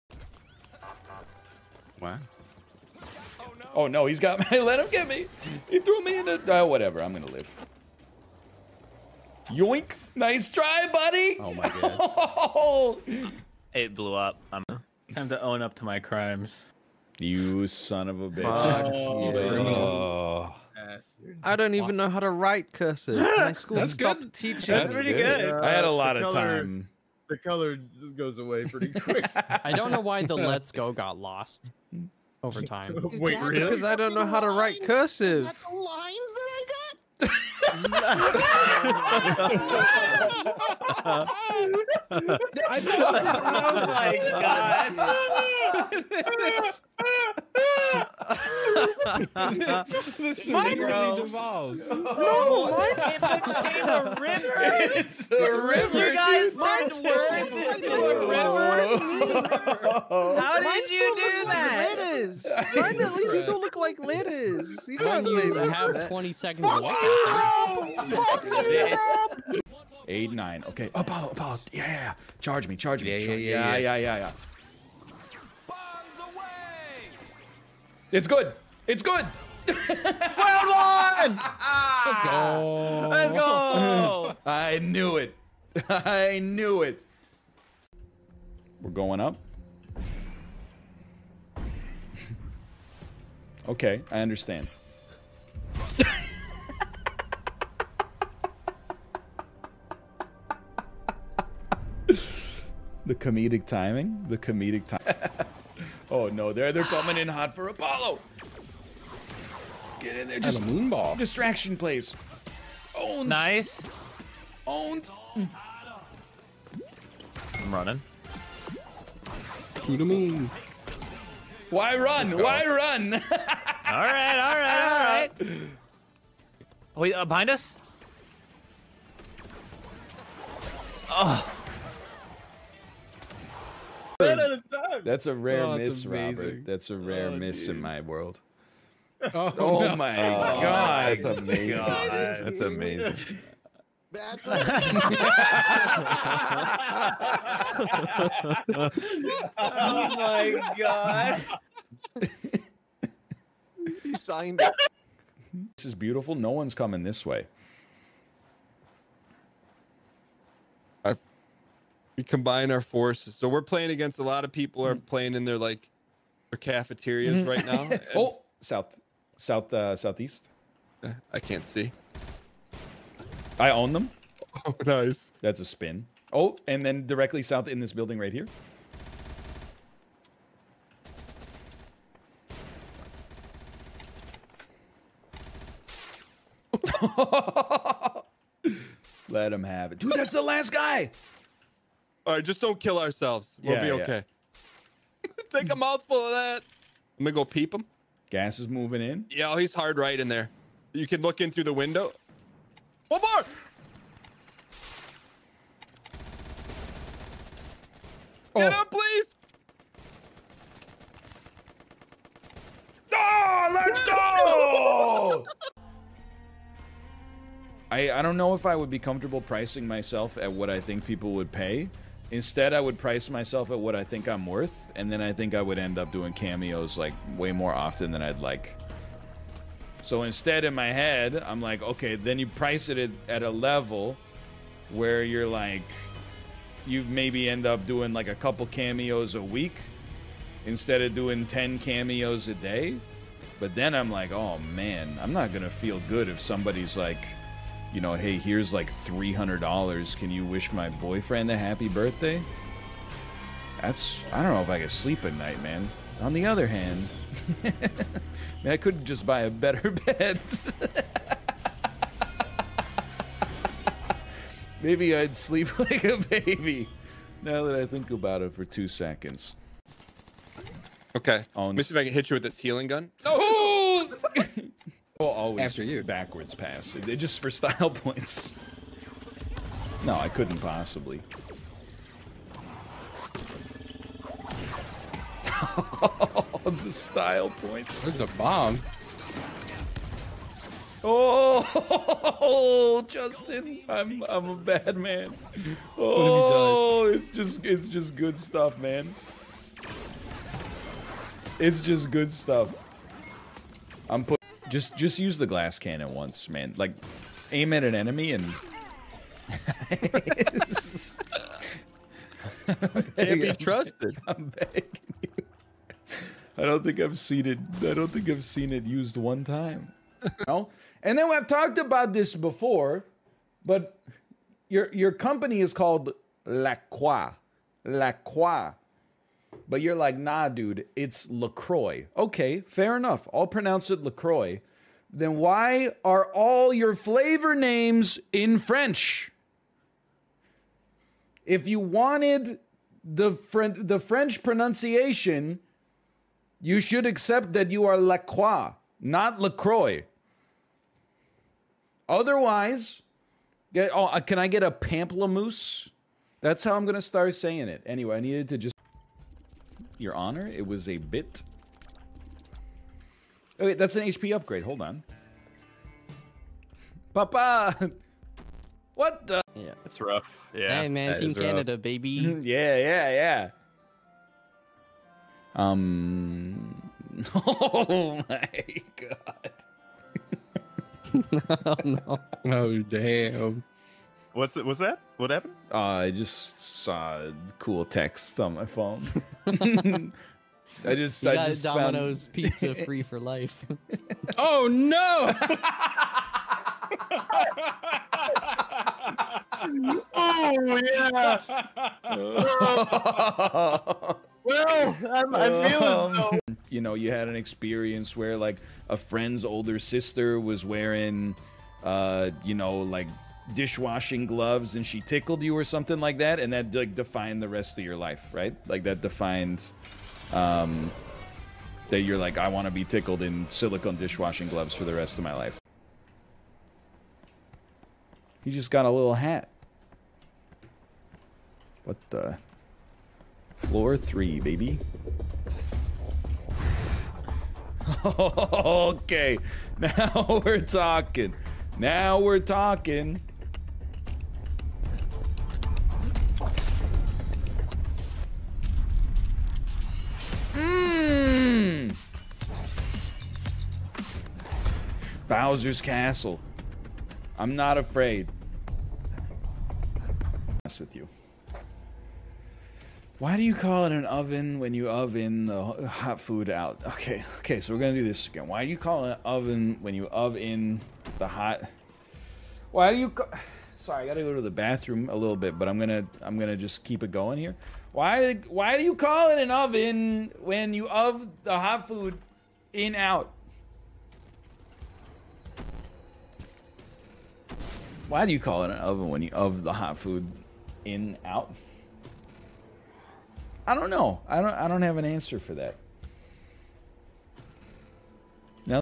NL has got the (dodge)balls | NL Twitch Highlights #48
00:00 - NICE TRY BUDDY  (Dani_Ren)
00:14 - Terminal Minimalism (Gray_Wing)
01:09 - Double KO Finish (DermitTheFregg)
01:27 - Ah yes (zakii)
01:44 - WHY RUN? (the_speakerforthedead)
02:14 - DEVOLVE (aspiringmage1)
02:35 - what tthe hell (MrHossie)
03:35 - Why Northernlion's not on Cameo (Percivol)
04:29 - healing gun (mttssz)
04:35 - Egg sends poor soul to the shadow realm (comatt6)
05:04 - Dan Dan the Cannon Man (DermitTheFregg)
05:20 - what's the deal with lacroix? (MossyQualia)
05:58 - HP up (AssistantWeebyWeeb188)
06:09 - Cool text (MysteryMister)
06:46 - Northernlion Self-reports (silvershi)
07:05 - He's the greatest gamer (misterrainbo)
07:40 - ? (AMPixel)